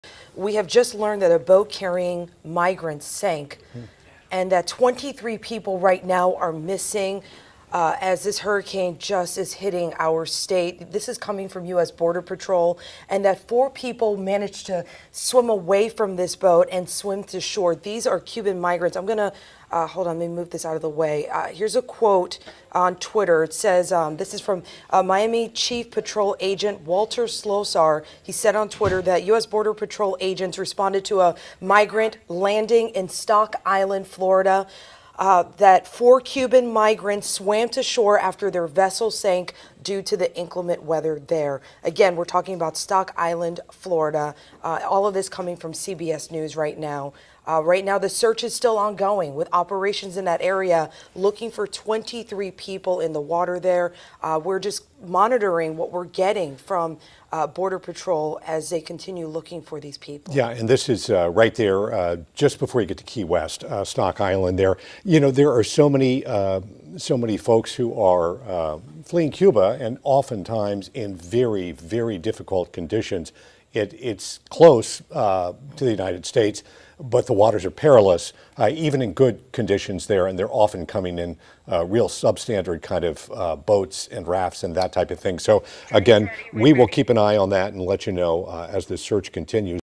23 migrants are missing off the coast of Florida
Twenty-three Cuban migrants are missing off the coast of Florida in the wake of Hurricane Ian, according to the U.S. Border Patrol. The government agency said four Cuban migrants swam to Stock Island after their vessel sank due to inclement weather.